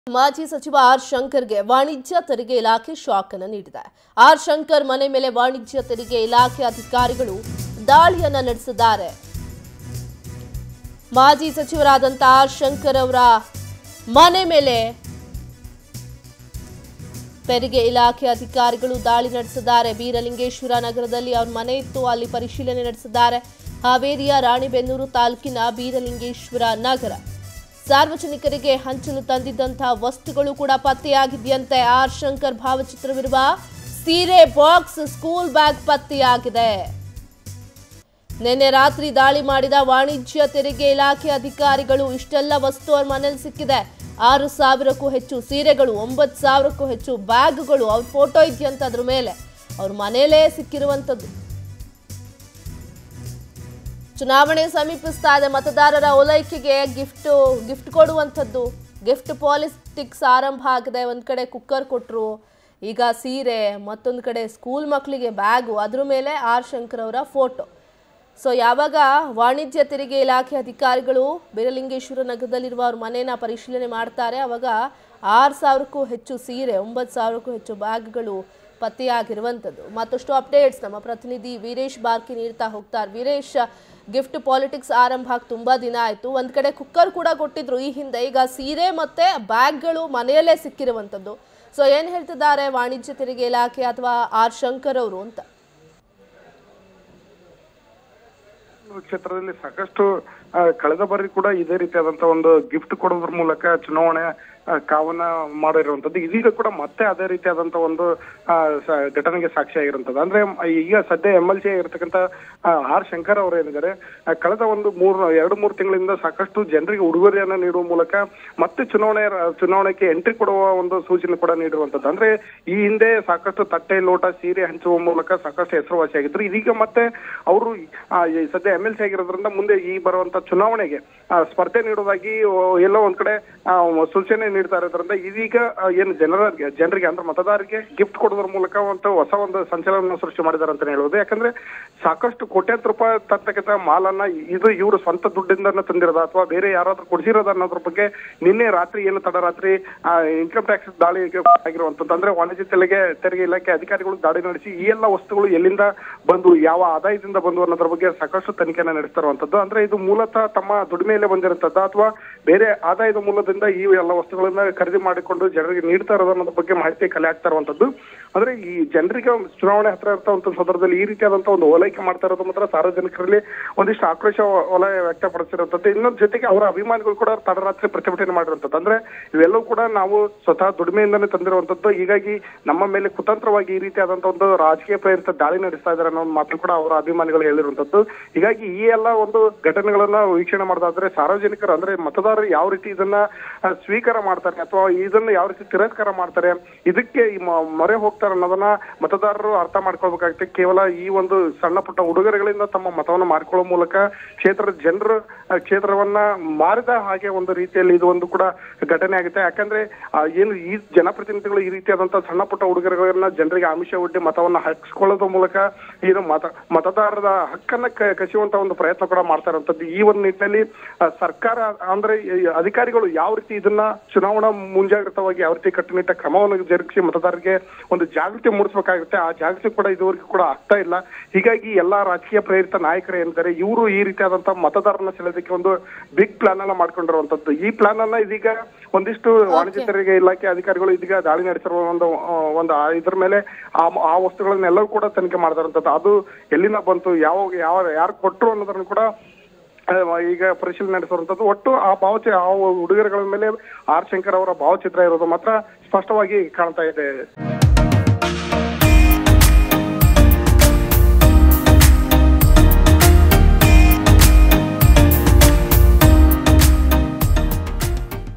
R Shankar : ಮಾಜಿ ಸಚಿವ ಆರ್‌ ಶಂಕರ್​ಗೆ ವಾಣಿಜ್ಯ ತೆರಿಗೆ ಇಲಾಖೆ ಶಾಕ್ | CTD​​​ | Power TV News
More Updates Keep Following Us